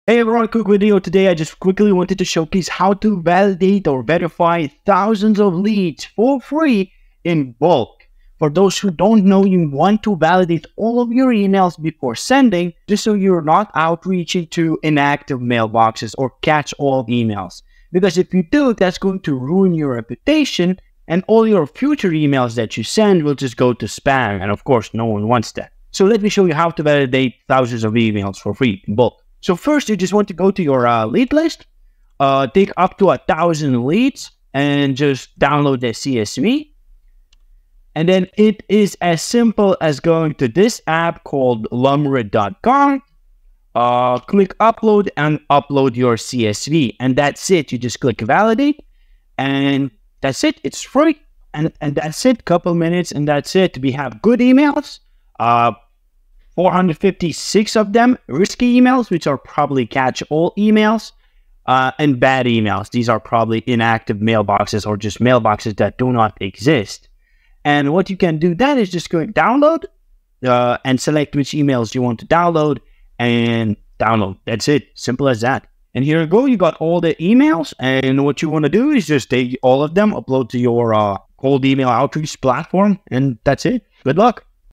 How I validate 1000s of emails for free in minutes
Quick video today, showcasing how you can verify thousands of emails for free, in bulk.

timestamps:
00:00 - How to validate thousands of emails for free in bulk
00:09 - Why do you need to validate emails
00:25 - Free email validation tutorial (validate emails in bulk for free)